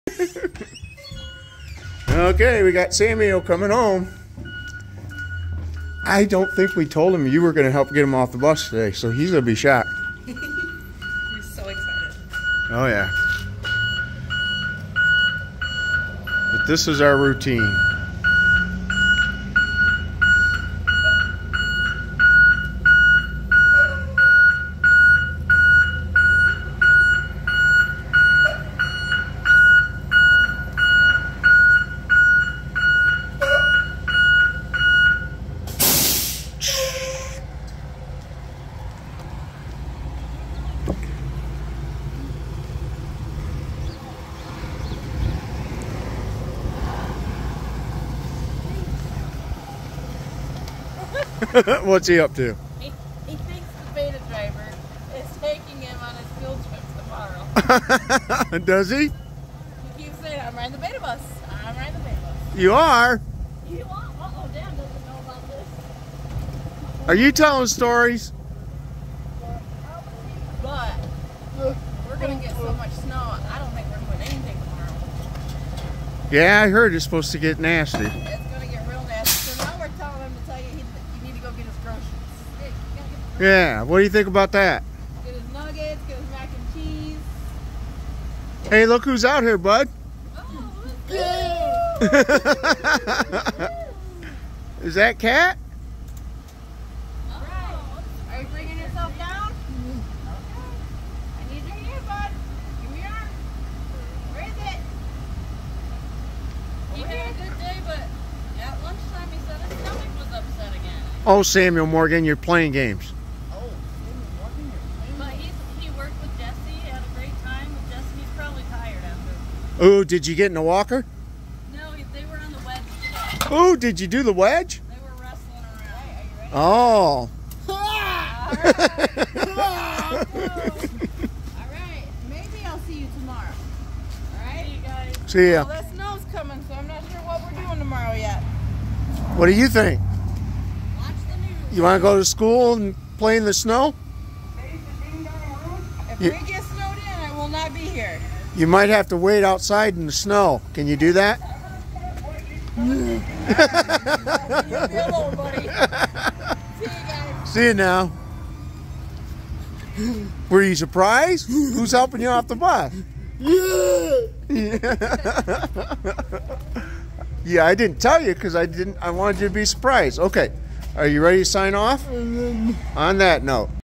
Sam getting home from school on the school bus 3.2.23
Sam getting off the school bus lift after school.